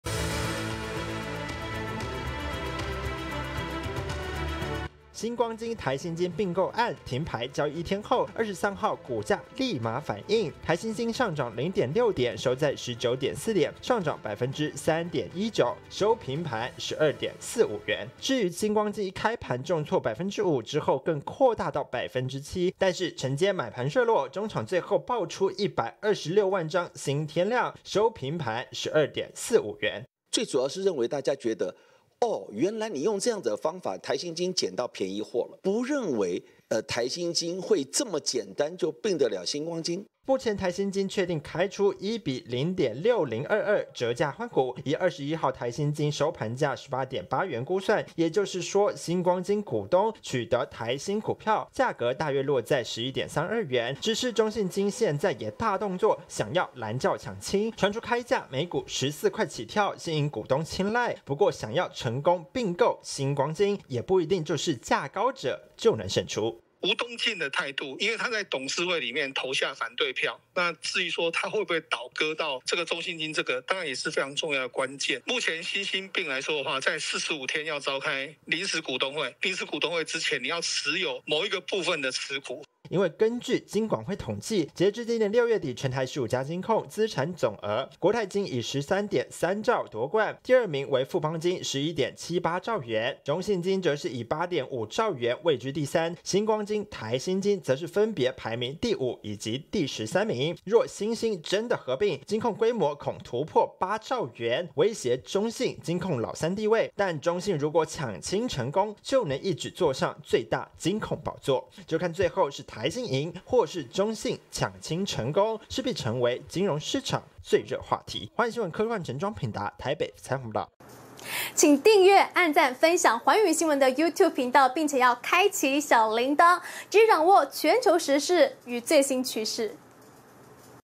「新新併」有變數？中信金「溢價」出手搶親｜寰宇新聞@globalnewstw
台新金、新光金併購案，牽動國內金融版圖位移引發話題，兩者停牌交易一天後，23日股價也反應話題，台新金漲幅百分之3左右，新光金則開盤就重挫，最後收平盤，而有意搶親的中信金，更傳出將開出14元高價，公開收購新光金股份，如果中信金搶親成功，就可一舉坐上國內最大金控的寶座。